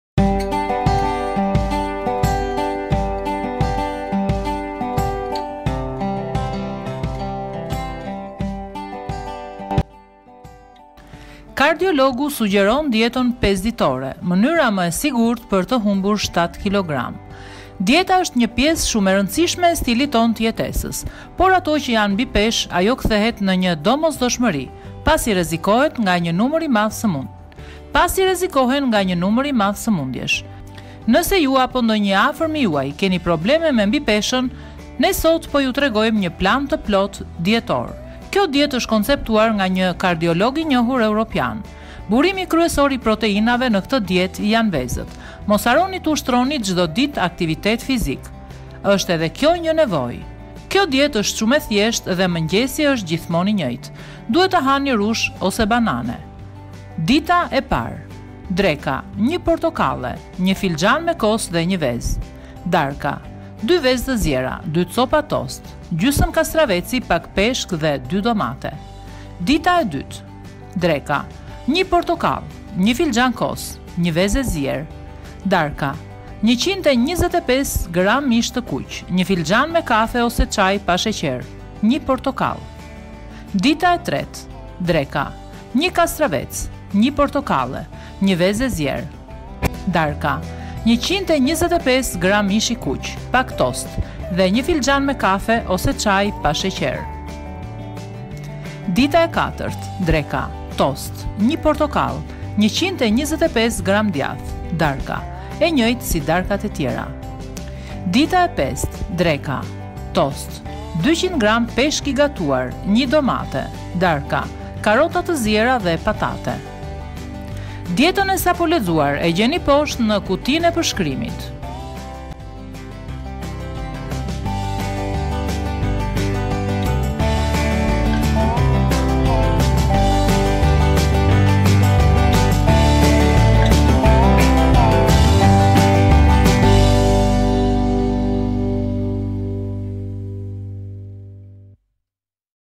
Kardiologu sugjeron dietën 5 ditore: Mënyra më e sigurtë për të humbur 7 kg
Kjo dietë është shumë e thjeshtë dhe mëngjesi është gjithmonë i njëjtë. Duhet të hani rrush ose banane.

Dita 1
Dreka: 1 portokalle, 1 filxhan me kos dhe 1 vezë;
Darka: 2 vezë të ziera, 2 copa tost, gjysmë kastraveci, pak peshk dhe 2 domate;

Dita 2
Dreka: 1 portokalle, 1 filxhan kos, 1 vezë e zierë;
Darka: 125 gramë mish të kuq, një filxhan me kafe ose çaj pa sheqer, një portokalle;

Dita 3
Dreka: 1 kastravec, 1 portokalle, 1 vezë e zierë;
Darka: 125 gramë mish i kuq, pak tost dhe një filxhan me kafe ose çaj pa sheqer;

Dita 4
Dreka: tost, 1 portokalle, 125 gr djathë;
Darka: e njëjtë si darkat e tjera;

Dita 5
Dreka: tost, 200 gr peshk i gatuar, 1 domate;
Darka: Karrota të ziera dhe patate;